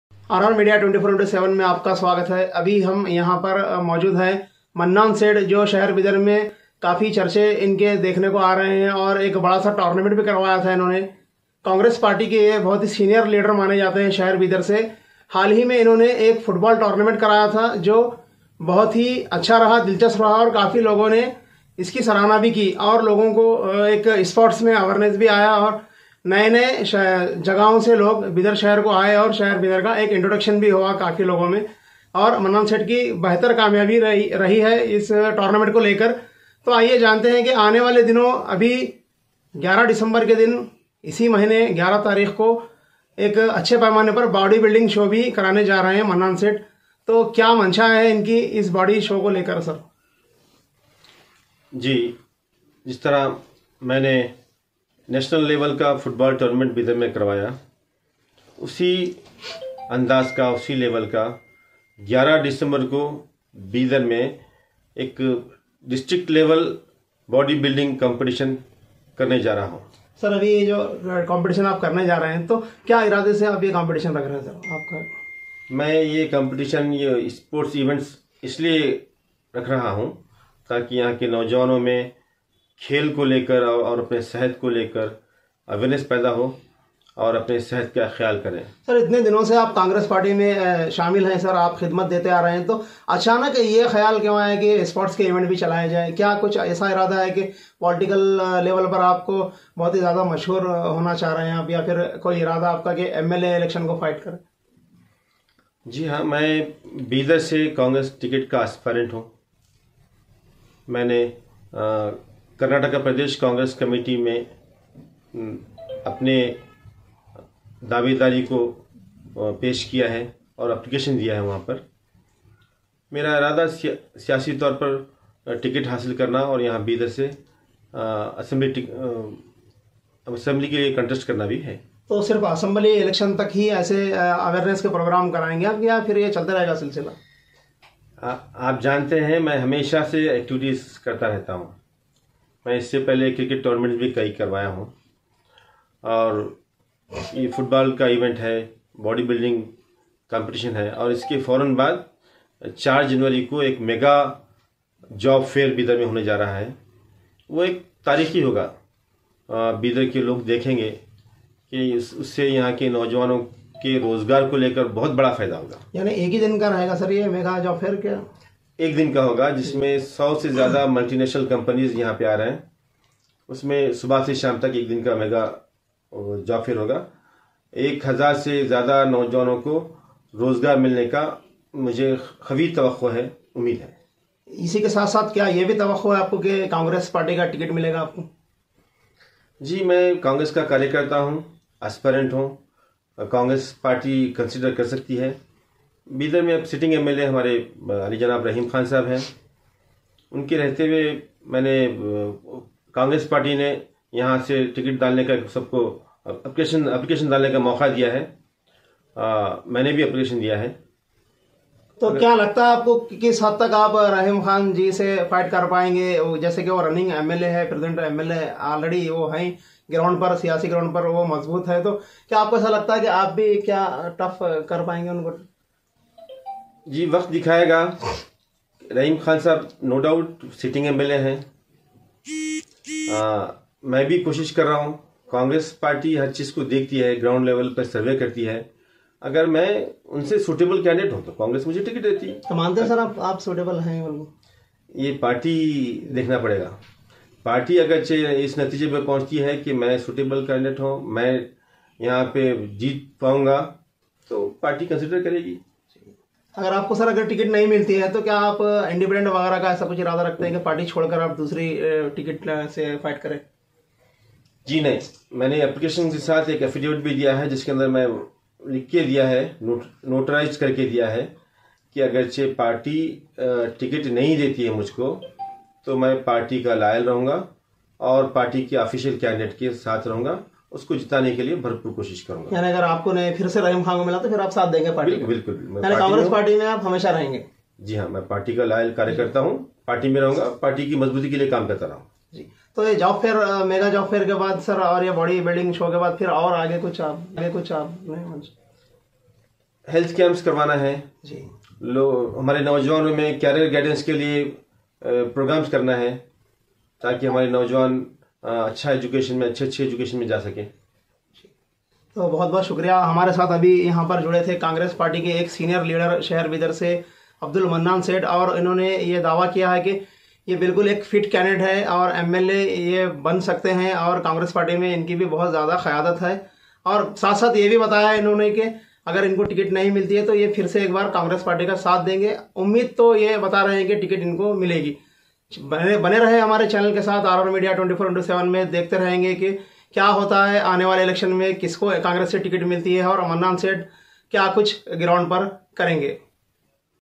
Abdul Mannan Sait Ne Ki R R MEDIA 24x7 Ke Samvadata Amjad Pasha Ke Sath Ki Khas Baat.
Aj Bidar Ke Senior Congress Leader Abdul Mannan Sait Ne Ki R R MEDIA 24x7 Ke Samvadata Amjad Pasha Ke Sath Ki Khas Baat.